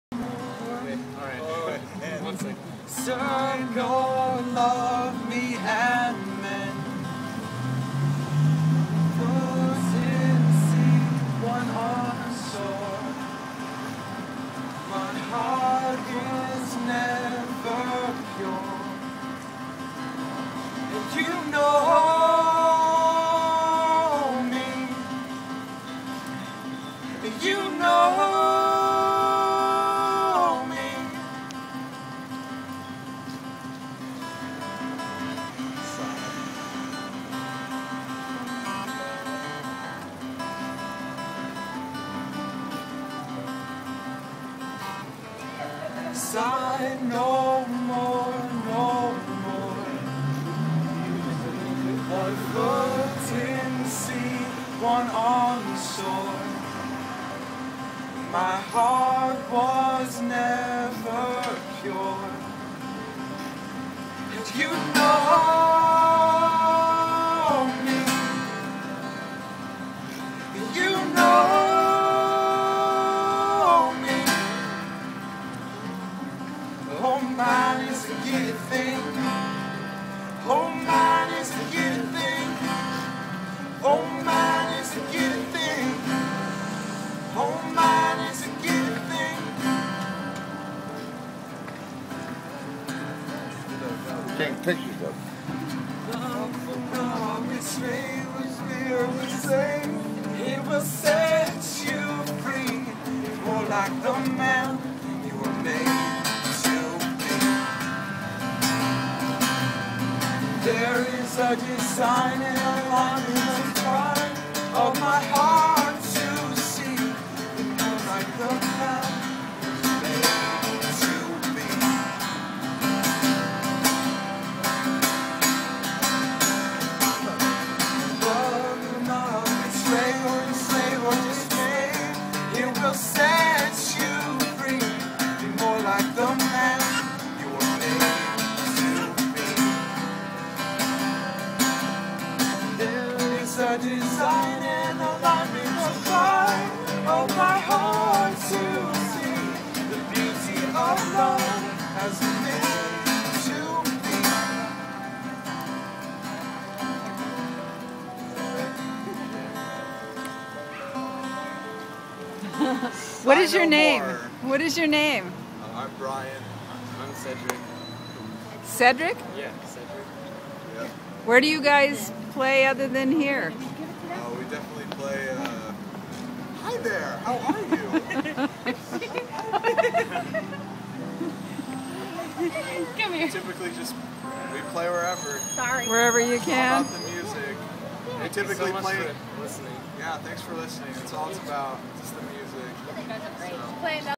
Walnut Creek Troubadours
2 Talented young men harmonize across from The Cheesecake Factory one evening last week.  This video is raw footage from my iPhone.  Sharon